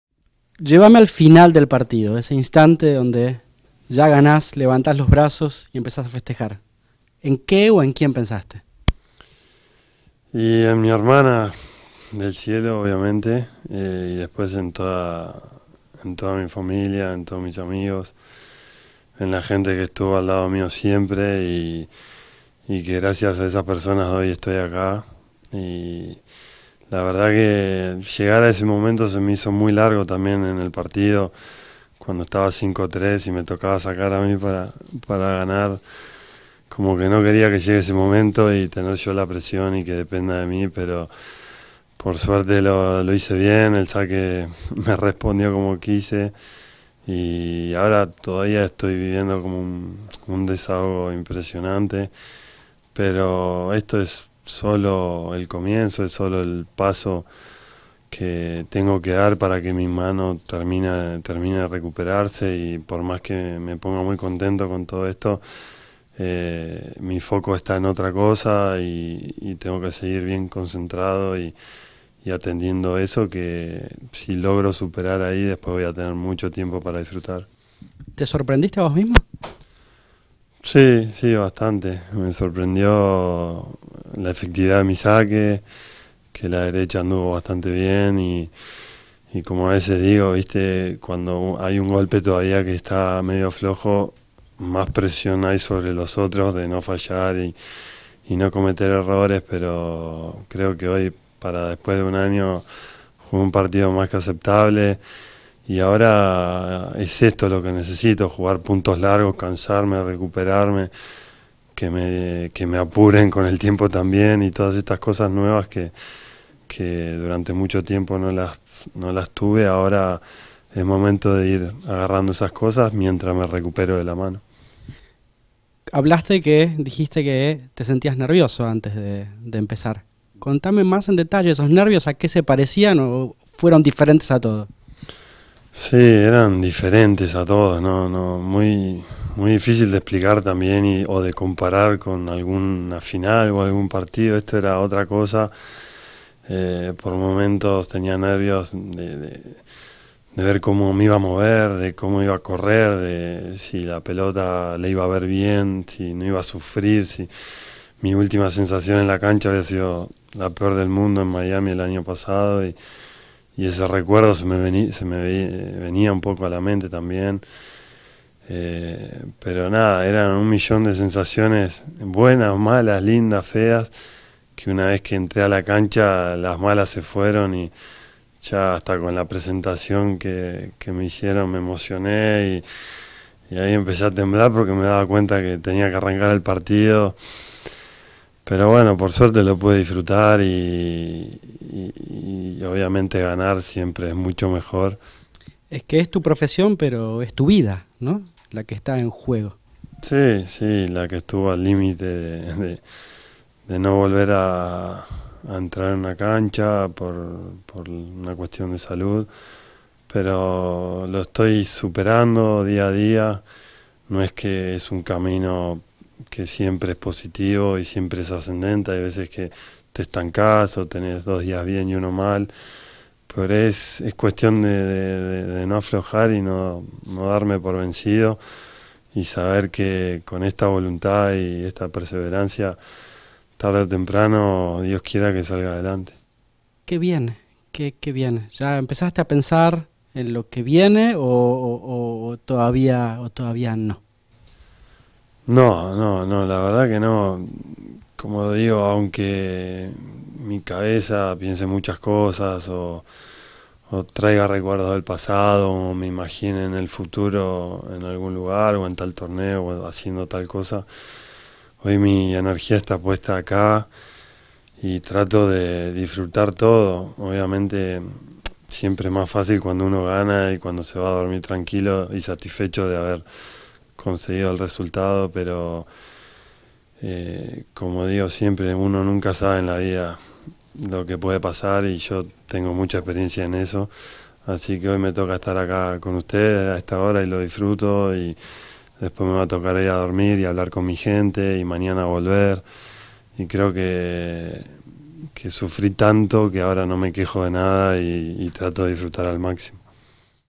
Juan Martín del Potro: "Este triunfo es solo el comienzo"
El argentino, Juan Martín del Potro, habló en exclusiva con Univision Deportes después de su triunfo ante Denis Kudla en el Delray Beach Open.